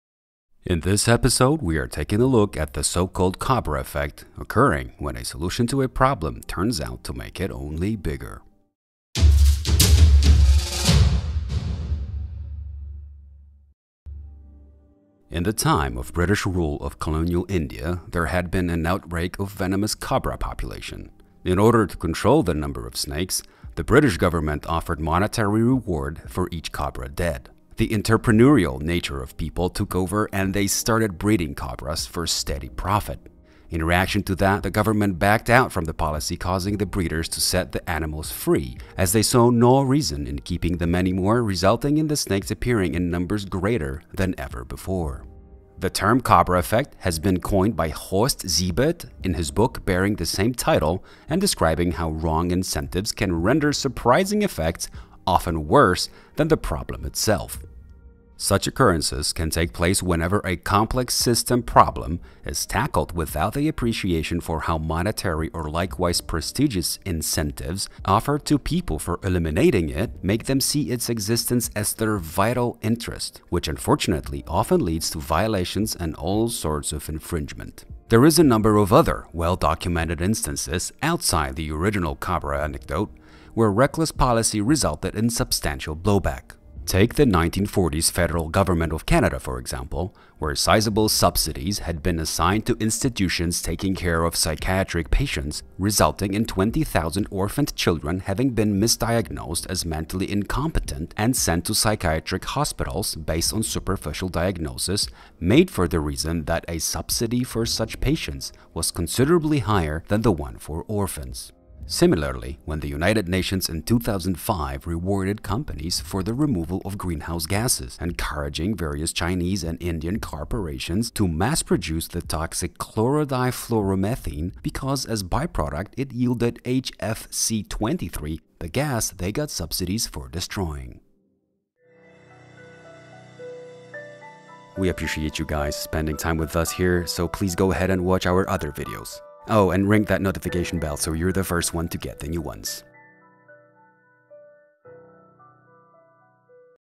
Cobra effect | 2 minute idea
What is so called "cobra effect" and why it often results in a solution being worse than problem itself?